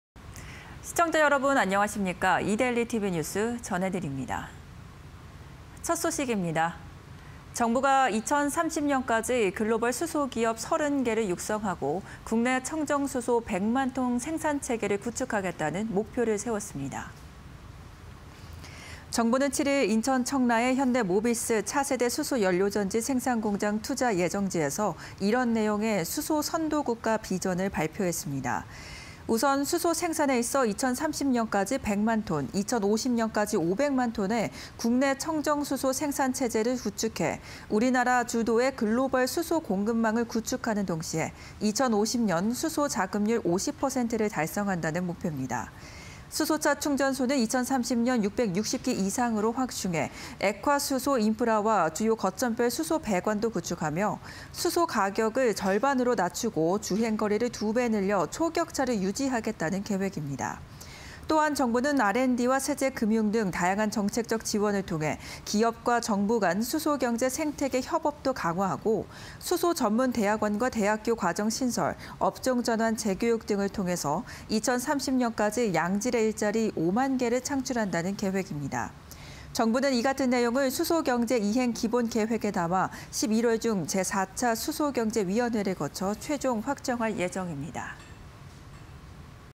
[이데일리N] 정부 "2030년 글로벌 수소기업 30개·청정수소 100만t 생산" (20211008)
정부가 2030년까지 글로벌 수소기업 30개를 육성하고 국내 청정수소 100만t 생산체제를 구축하겠다는 목표를 세웠습니다.
정부는 7일 인천 청라의 현대모비스 차세대 수소연료전지 생산공장 투자 예정지에서 이런 내용의 '수소선도국가 비전'을 발표했습니다.
우선 수소생산에 있어 2030년까지 100만t, 2050년까지 500만t의 국내 청정수소 생산체제를 구축해 우리나라 주도의 글로벌 수소 공급망을 구축하는 동시에 2050년 수소 자급률 50%를 달성한다는 목표입니다.
수소차 충전소는 2030년 660기 이상으로 확충해 액화수소 인프라와 주요 거점별 수소배관도 구축하며, 수소차 가격을 절반으로 낮추고 주행거리를 2배 늘려 초격차를 유지하겠다는 계획입니다.
또한 정부는 R&D·세제·금융 등 다양한 정책적 지원을 통해 기업과 정부 간 수소경제 생태계 협업도 강화하고, 수소 전문 대학원·대학교 과정 신설, 업종전환 재교육 등을 통해 2030년까지 양질의 일자리 5만개를 창출한다는 계획입니다.
정부는 이 같은 내용을 '수소경제이행기본계획'에 담아 11월 중 제4차 수소경제위원회를 거쳐 최종 확정할 예정입니다.